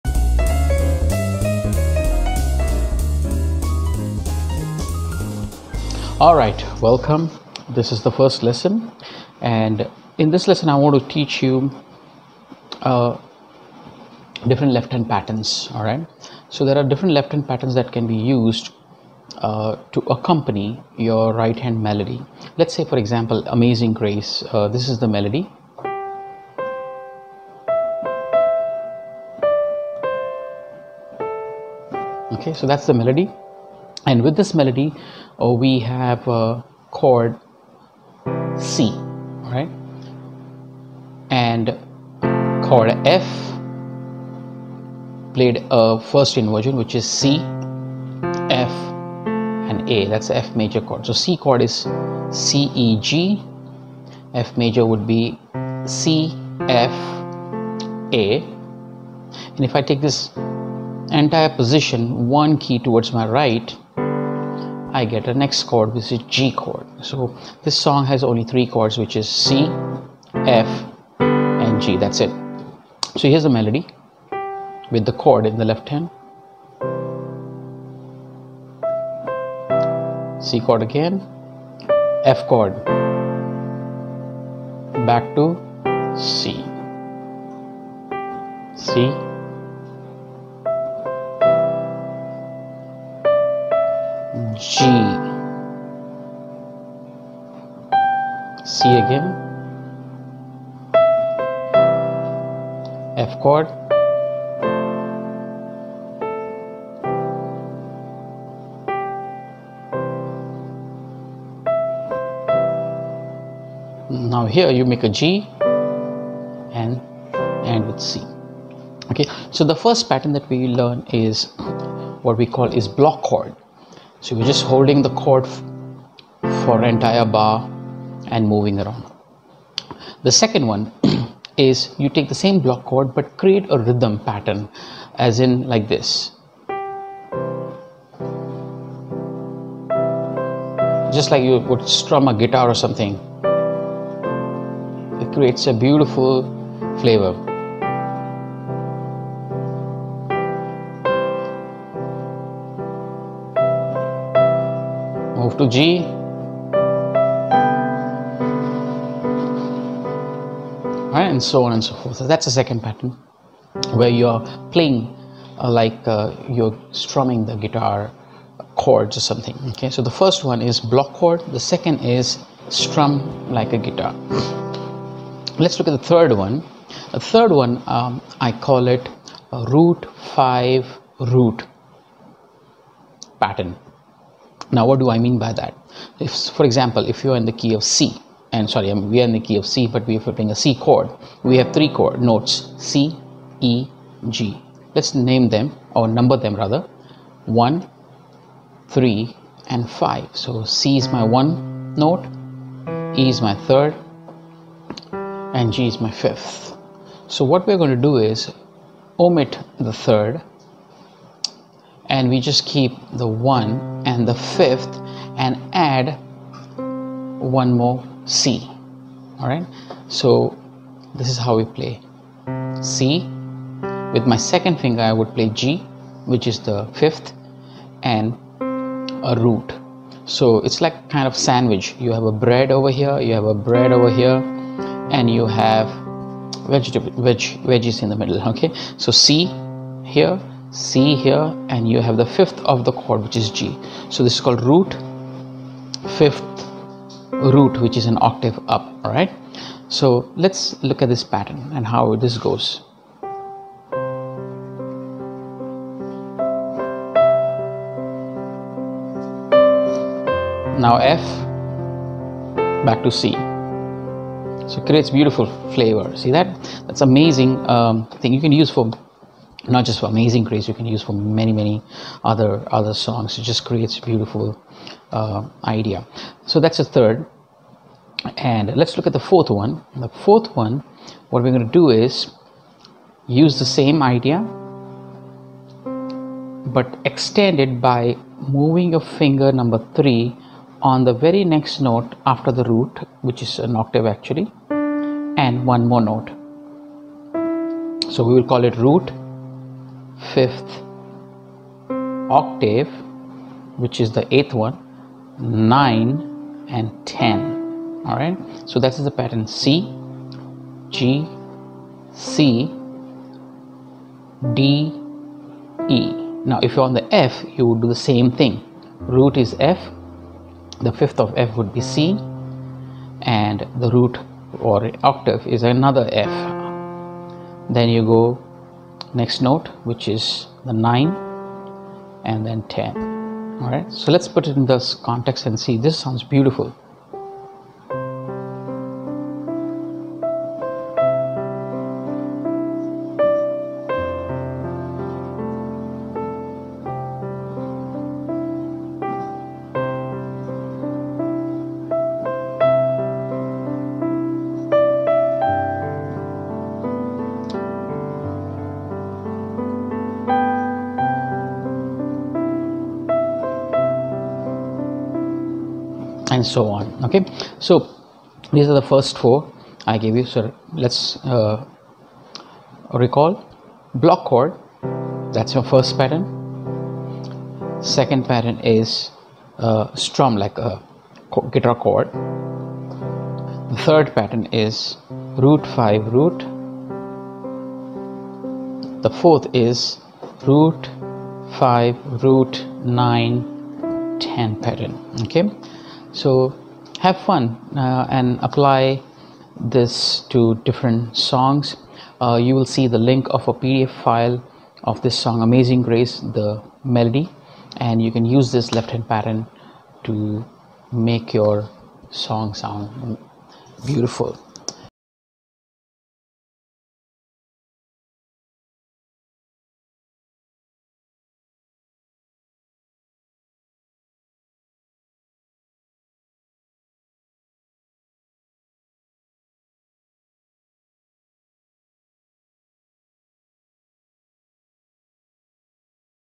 Part 1 : 4 Amazing Left Hand Piano Patterns
Here are four awesome left hand patterns to play with any song! Spice up your playing with these techniques!